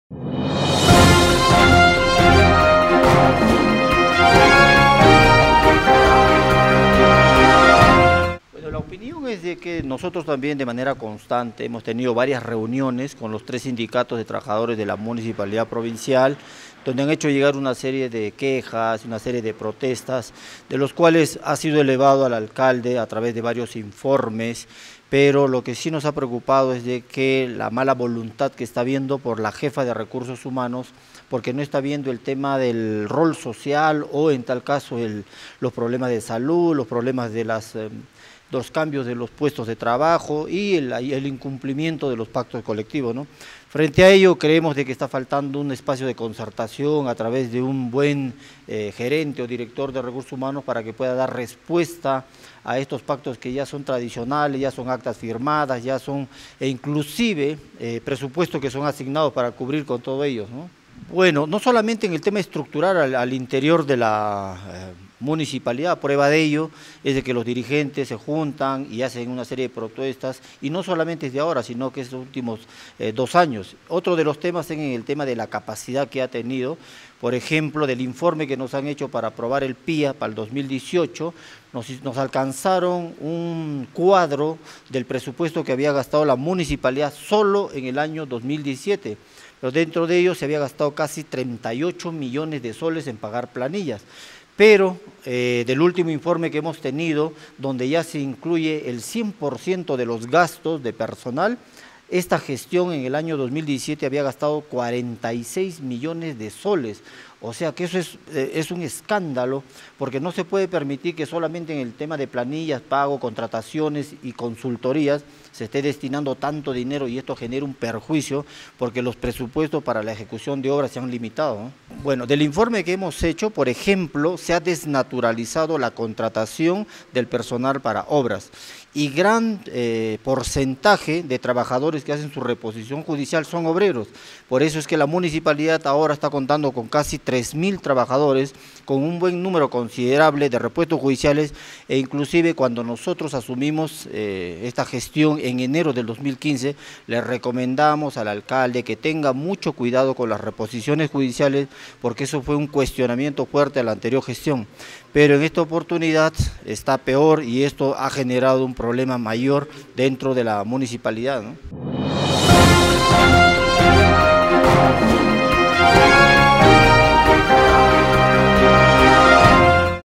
CUESTIONA A GERENTE DE RECURSOS HUMANOS, POR NO IMPLEMENTAR PACTO COLECTIVO
DANIEL ABARCA - REGIDOR DE LA MUNICIPALIDAD DEL CUSCO

SEGÚN EL REGIDOR DANIEL ABARCA, EL MALESTAR DE LOS SERVIDORES EDILES SE GENERA PORQUE LA GERENCIA DE RECURSOS HUMANOS NO TIENE LA VOLUNTAD DE HACER CUMPLIR EL PACTO COLECTIVO, PESE A CONTAR CON EL PRESUPUESTO NECESARIO …